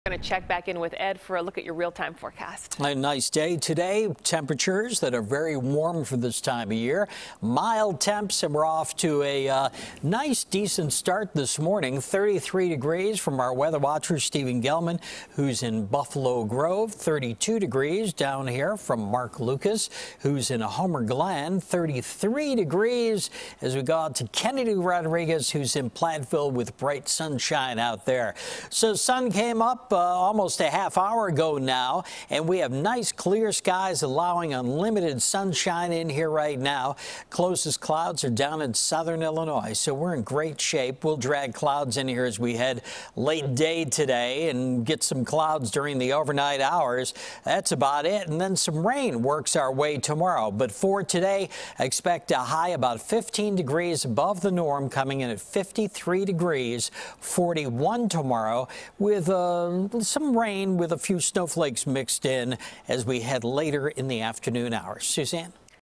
Warm Sunday Before Rain And Snow During The Week
CBS 2 Meteorologist Ed Curran has the 7 a.m. forecast for Sunday, Feb. 23, 2020.